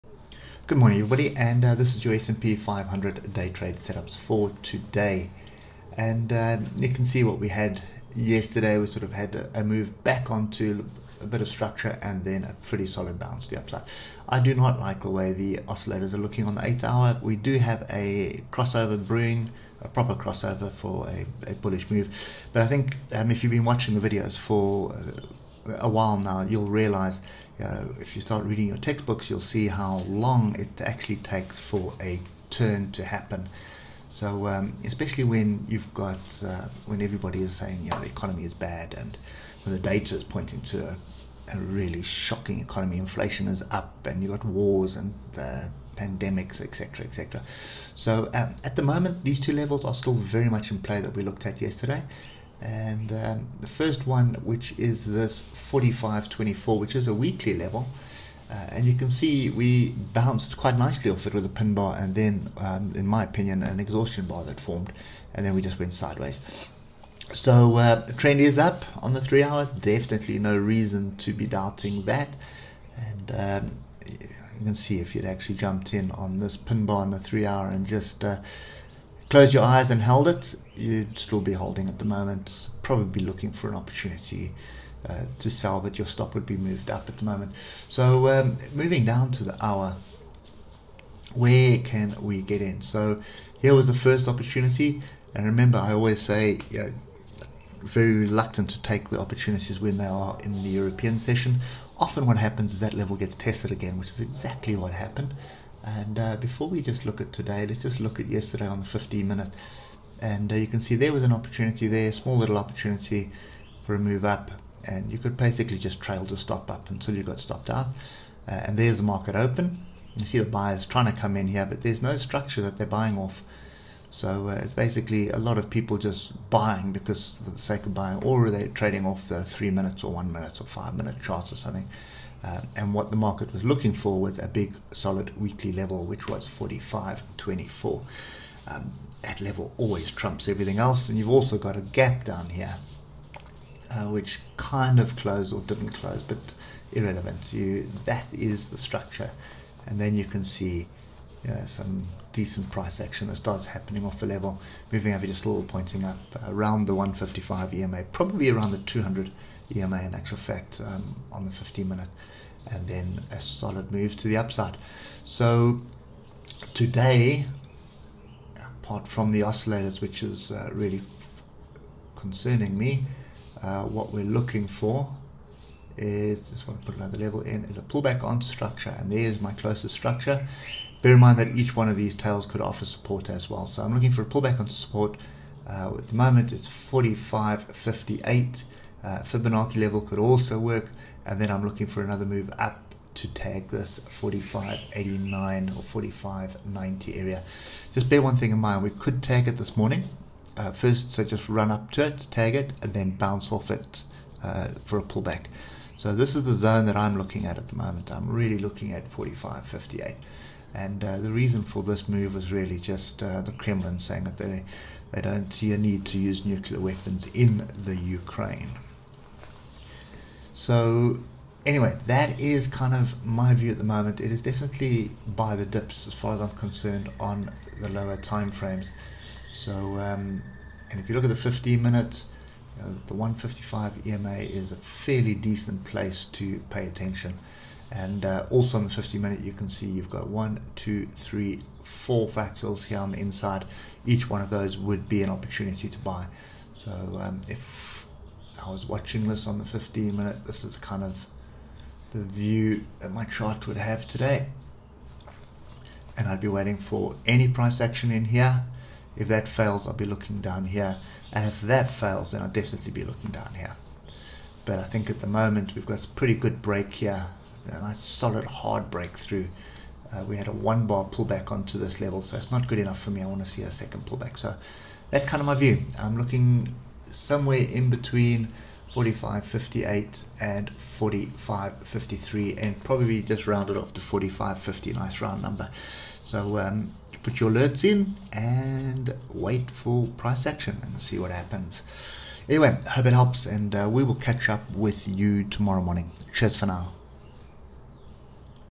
S&P 500 Today 29 March 2022 Technical Analysis. Daily market analysis
Daily Technical Analysis of S&P 500 Today 29 March 2022
Daily market analysis of the S&P 500,  a stock market index tracking the stock performance of 500 of the largest companies listed on stock exchanges in the United States.